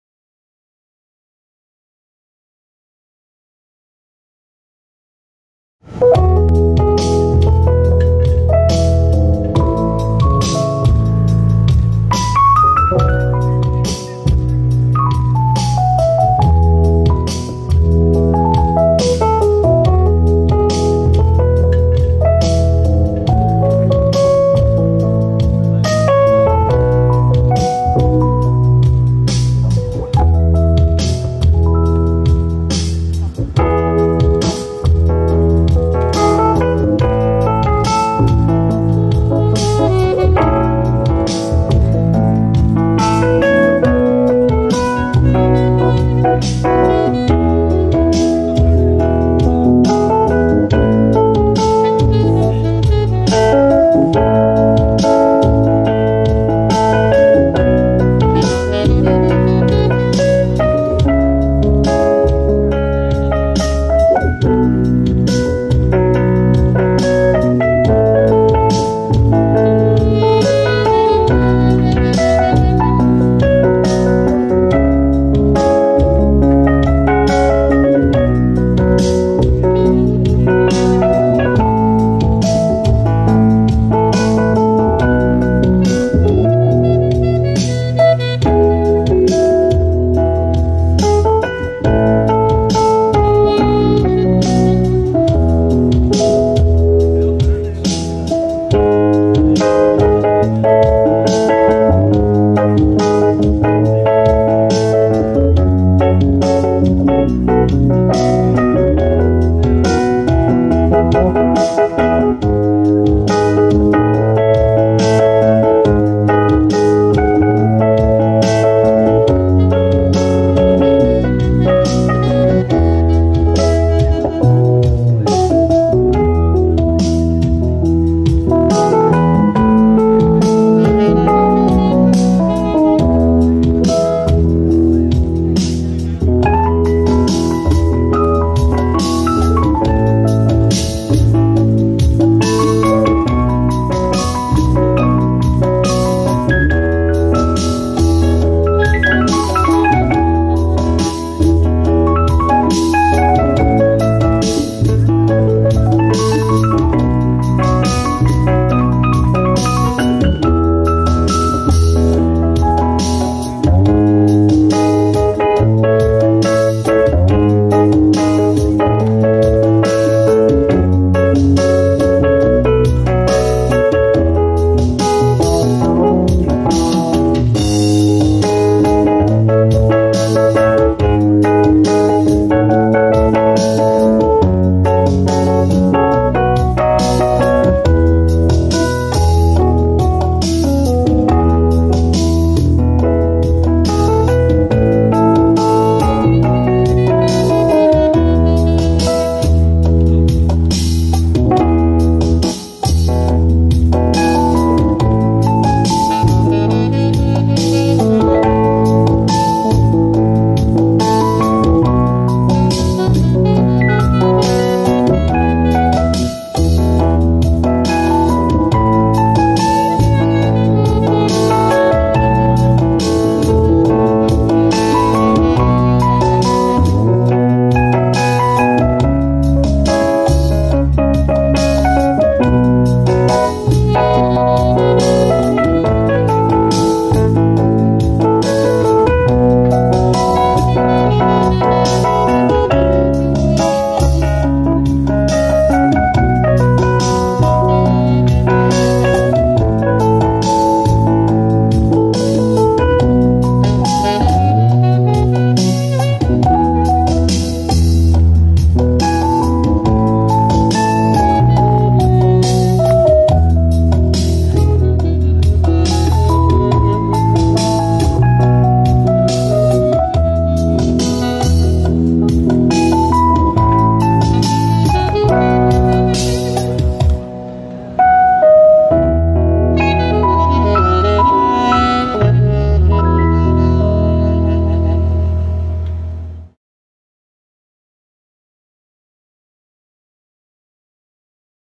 Bollywood Tinto
Instrumental lounge music trio onboard Deltin Royale casino cruise liner in Goa